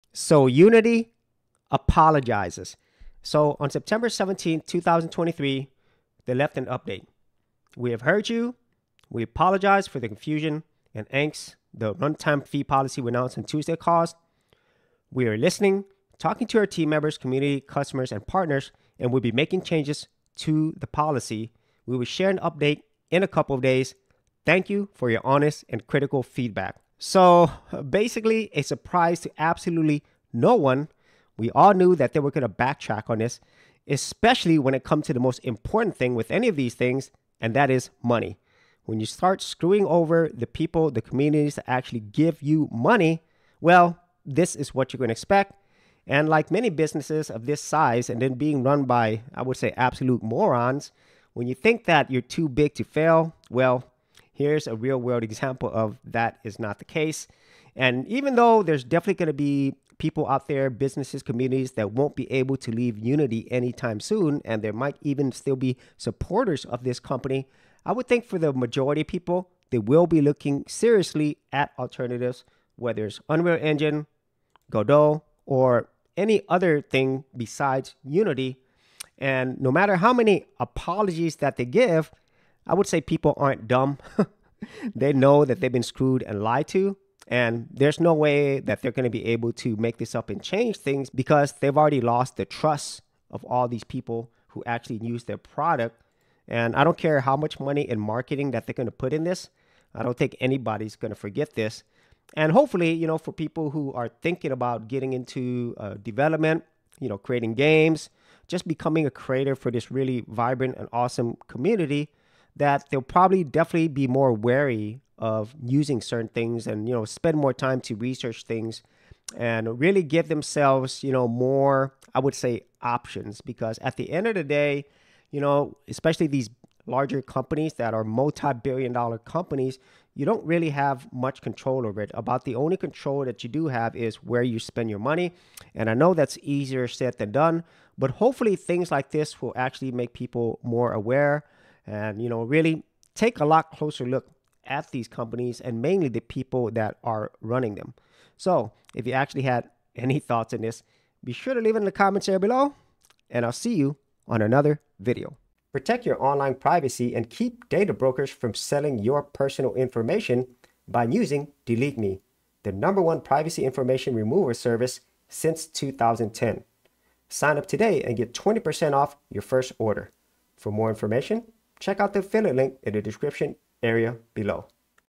Unity “Apologizes”…🤣🤣🤣
The Unity Engine is in Trouble and Developers are not happy!

***Get Social***